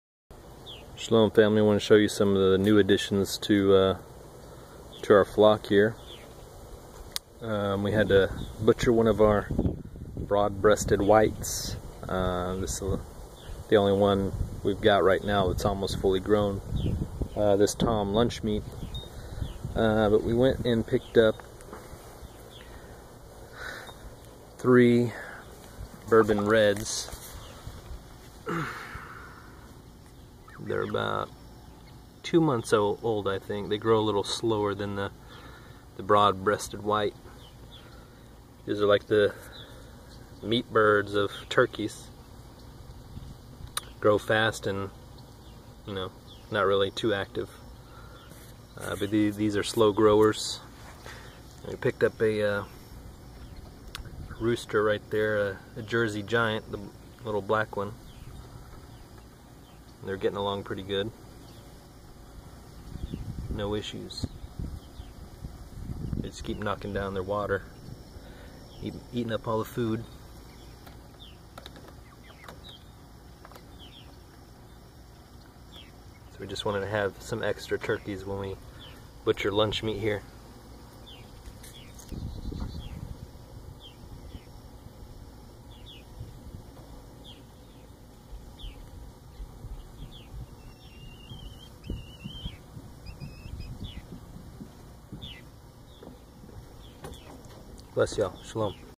Additions to the flock
3 Red Bourbon Turkeys & Jersey Giant Rooster